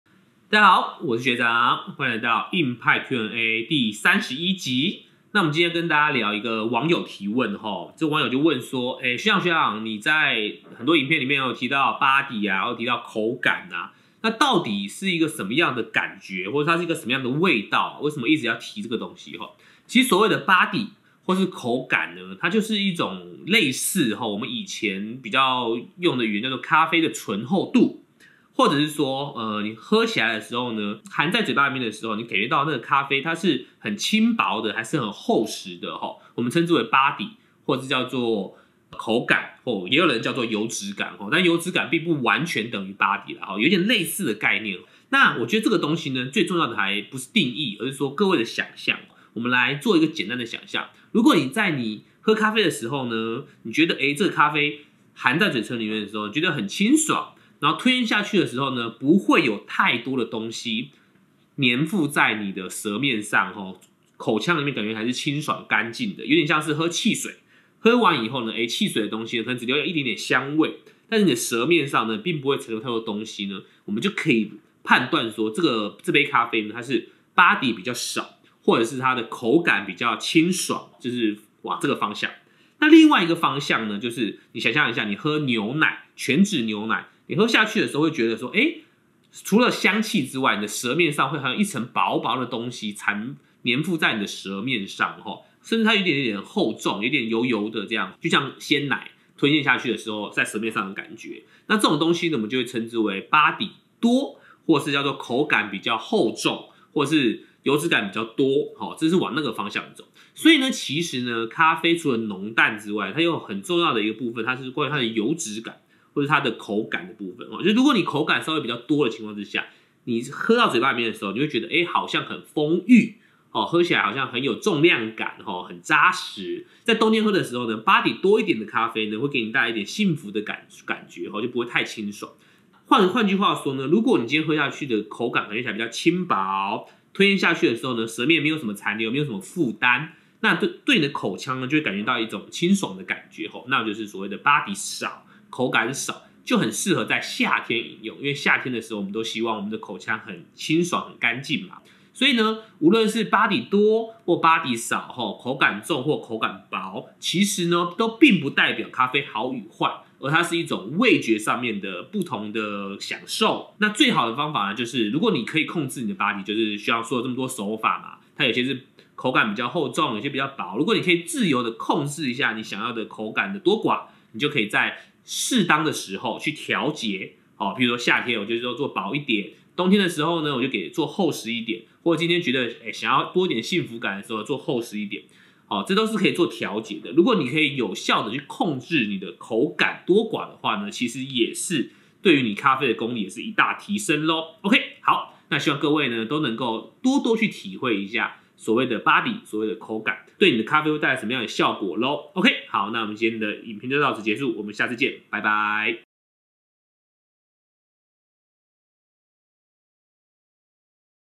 硬派90秒_#31_Body、口感是什麼意思？
常常我們在看手沖咖啡的風味敘述的時候
都會看到一些對於 BODY、口感的形容
那到底代表著什麼意思呢？